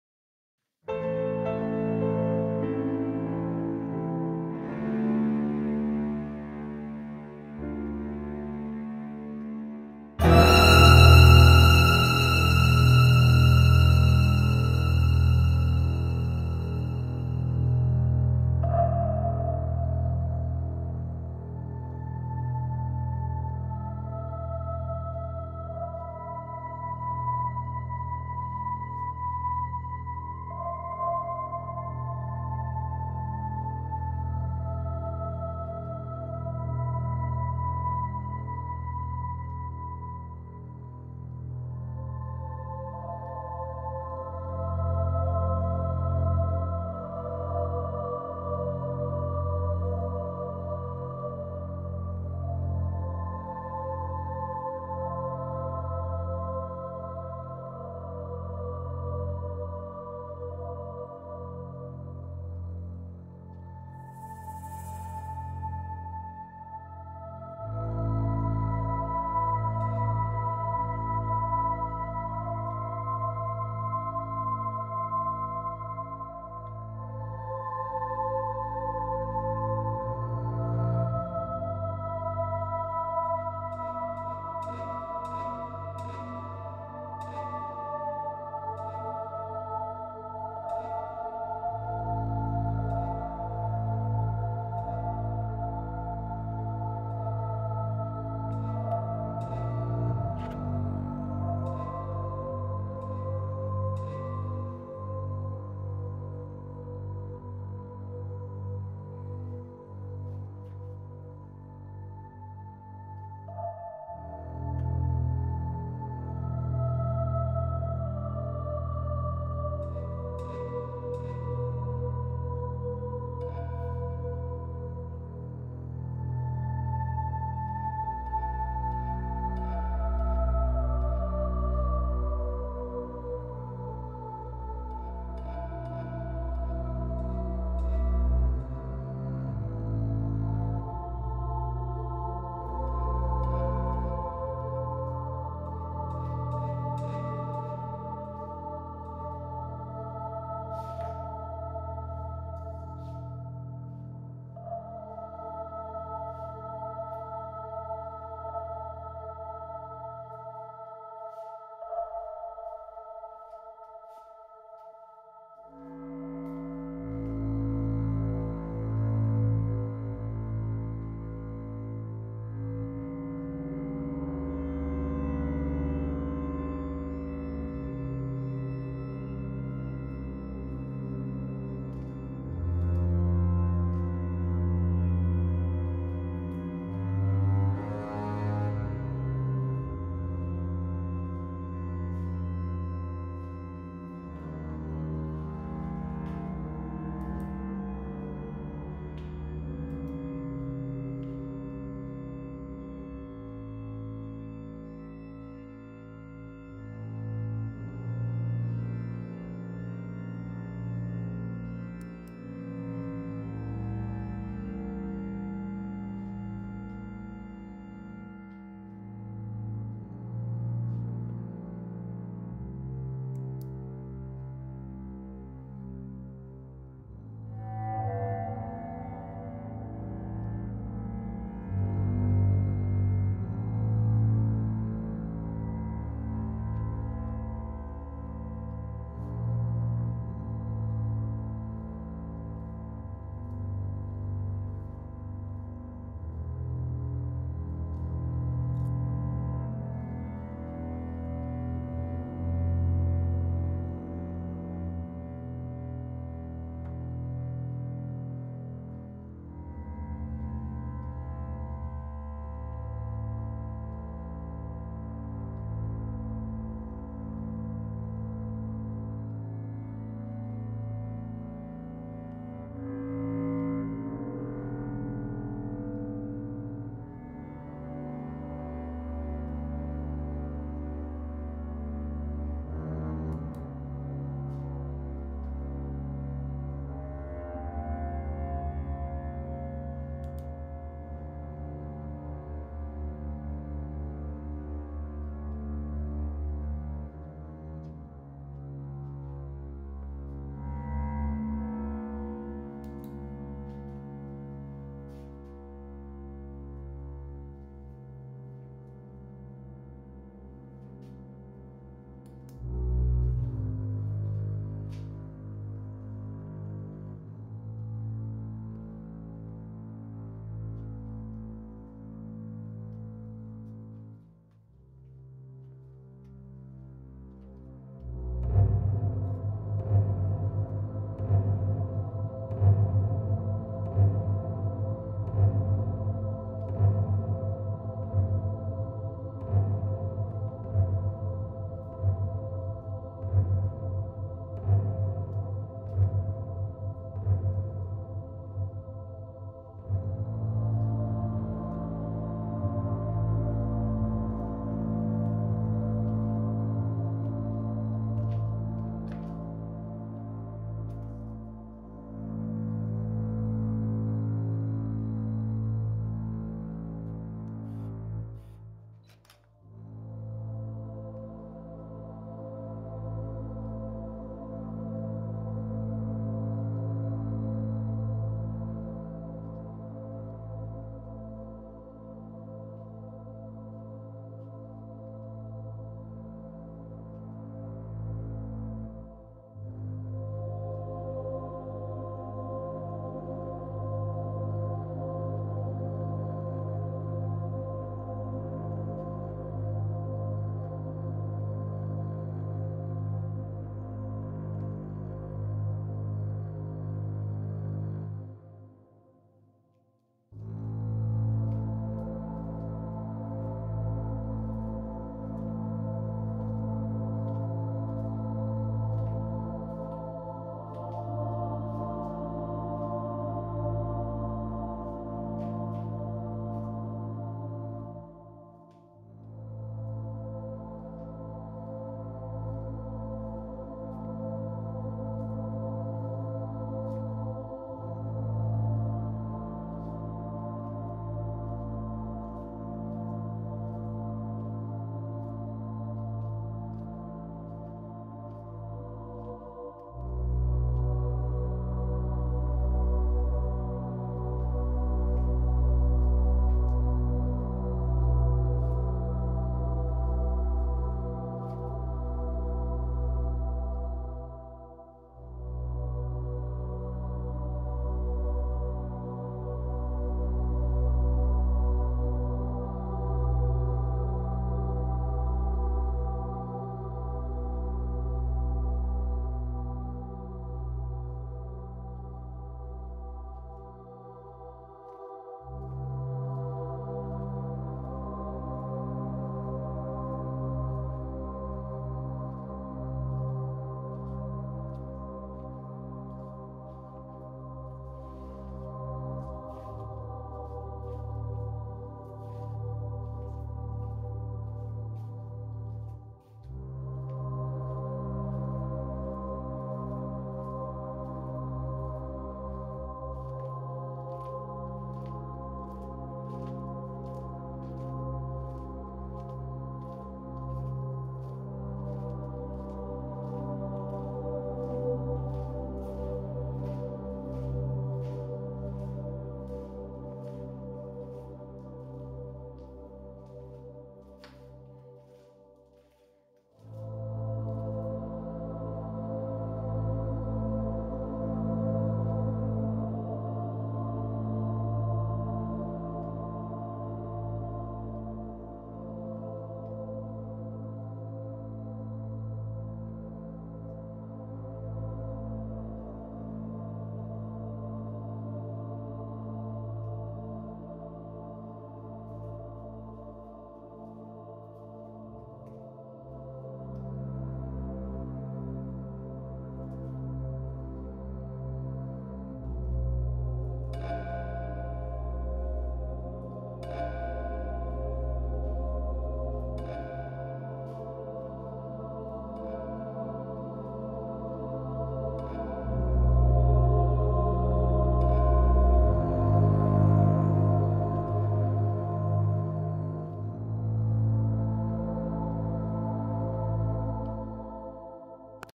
Exploring Abandoned Hospital in 360
Exploring an abandoned hospital connected to an old Asylum. Built in the 1930's this building acted as a hospital for the asylum for many years. Among other things Lobotomies took place somewhere in this building. It was later converted to offices for the university, and has sat abandoned since 2012. It now sets awaiting demolition most likely having already been demolished by the time this video posts. This is a 360 video of the first floor.